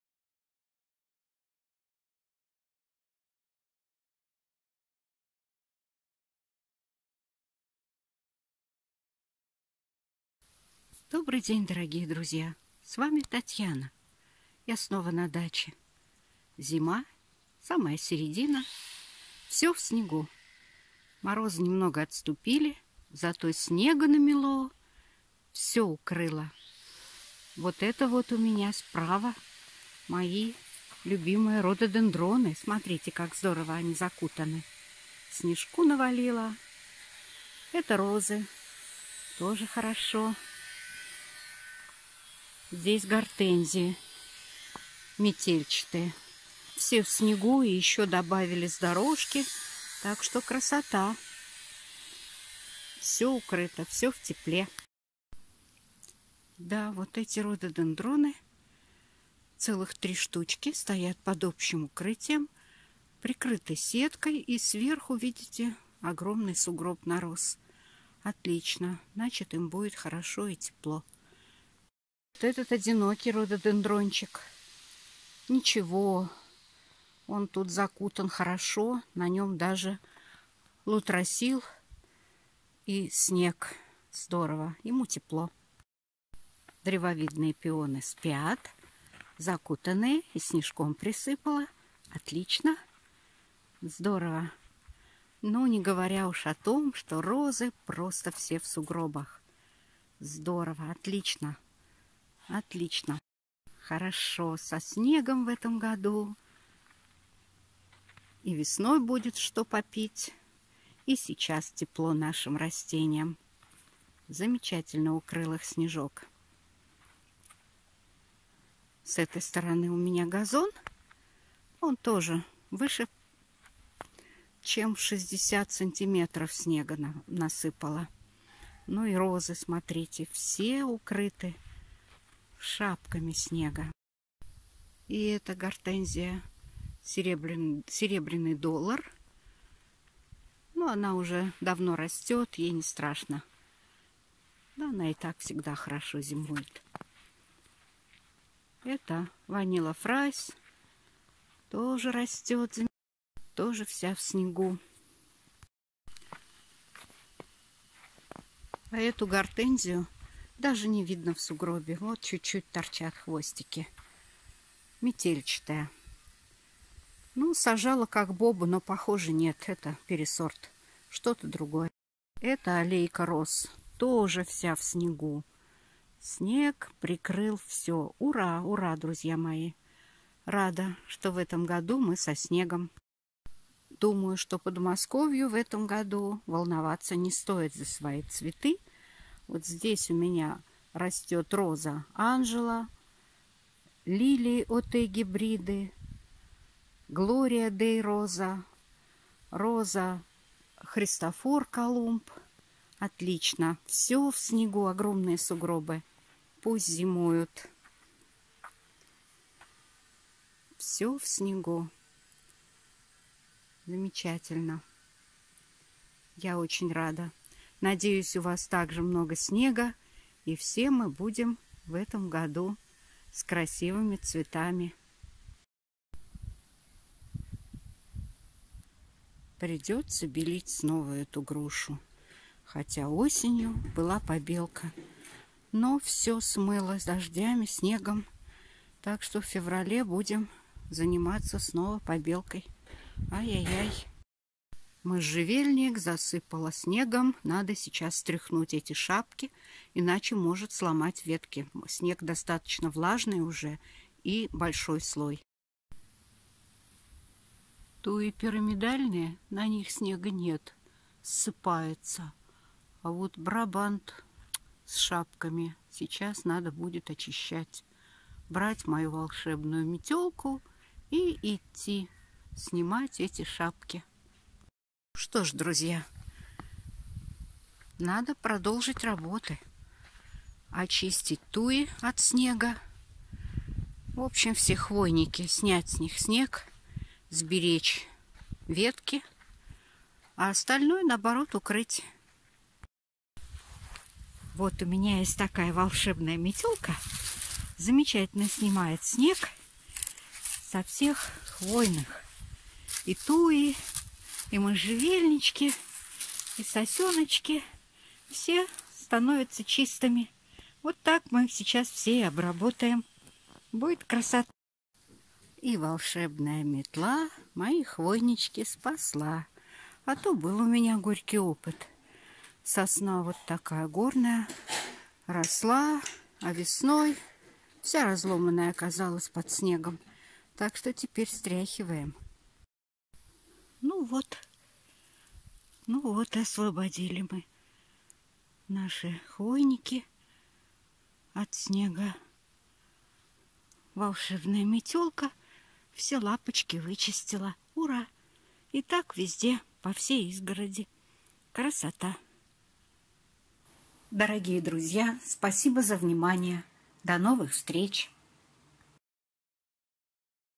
Хвойные растения зимой.
Зима. Наши розы отдыхают под укрытием. В этом коротком видео я покажу, как ухаживать за хвойными растениями зимой. Нужен ли им снег? Нет, все хвойные нужно освободить от налипшего на них снега, иначе не избежать печальных потерь красивых лапок.
Посмотрите, как я это делаю. Все просто.